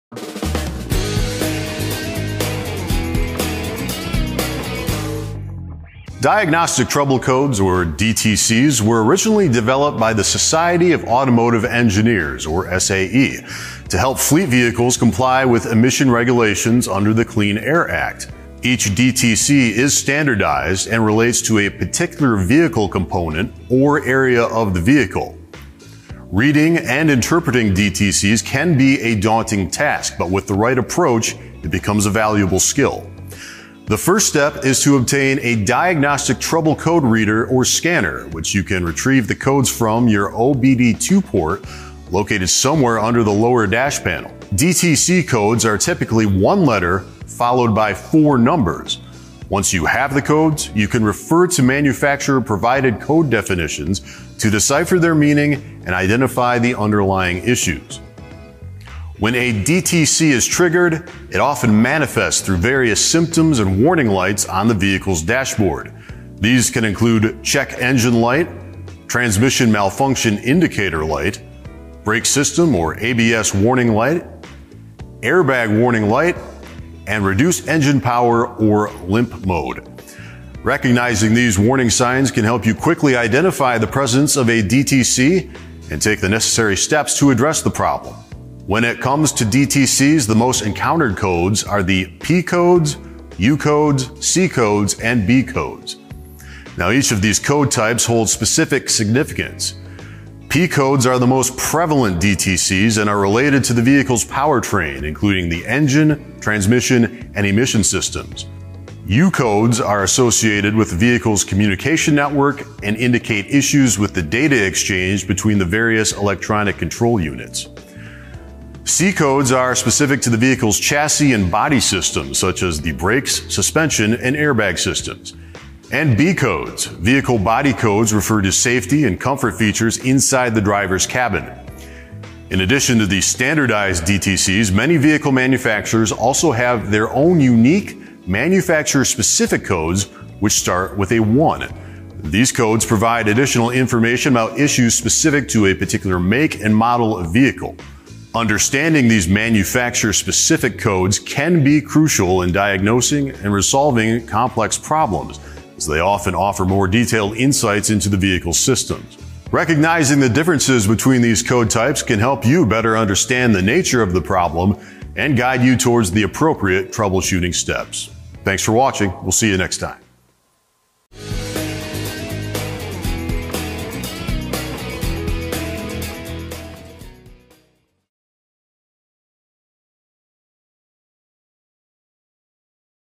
What Does That DTC Mean?
Discover the different types of DTCs, including P, U, C, and B codes, and how they help pinpoint problems with your customer's engine, transmission and more.